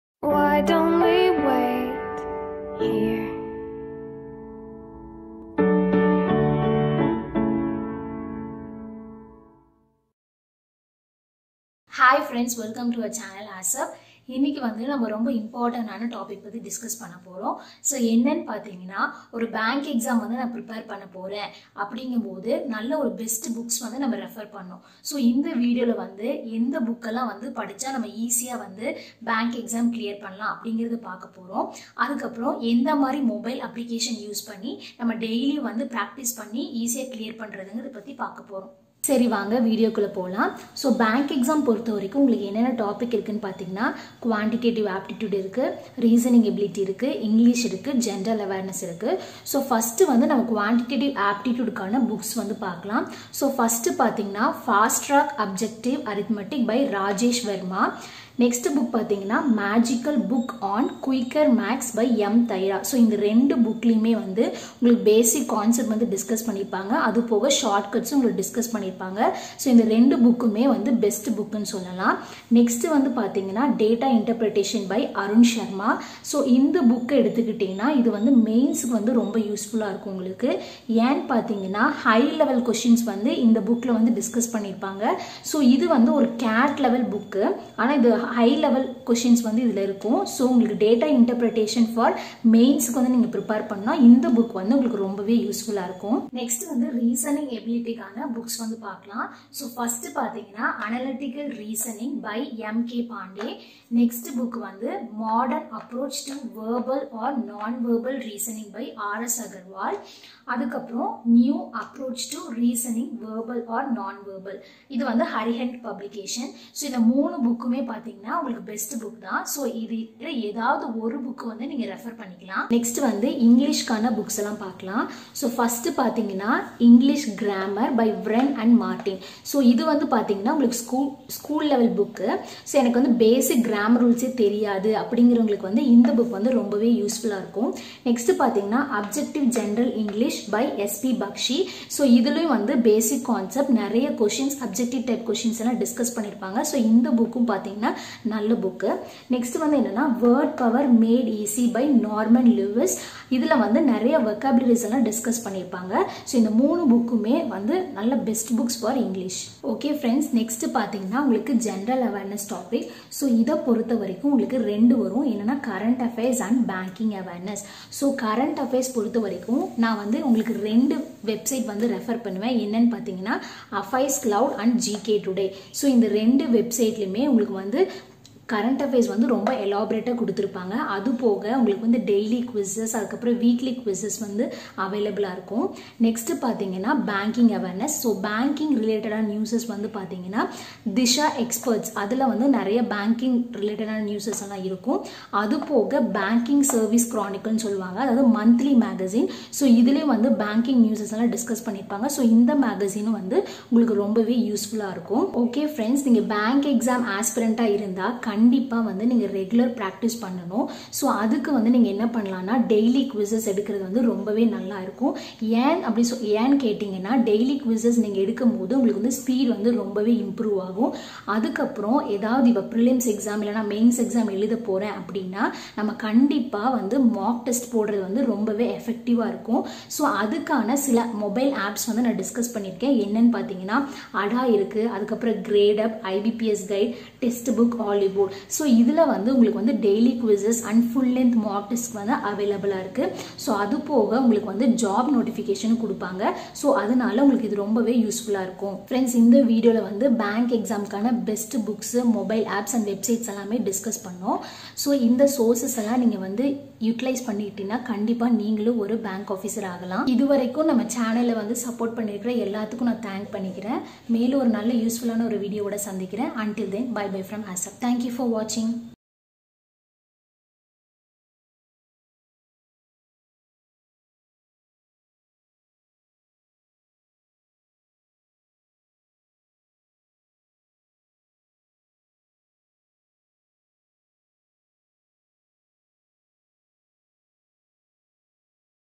Best Books For Banking Exam in tamil | Best Books for IBPS, SBI | ASAP Goverment Jobs Tamil
In this video we provided the best books for bank exam preparation and also we provided  the mobile apps for daily practic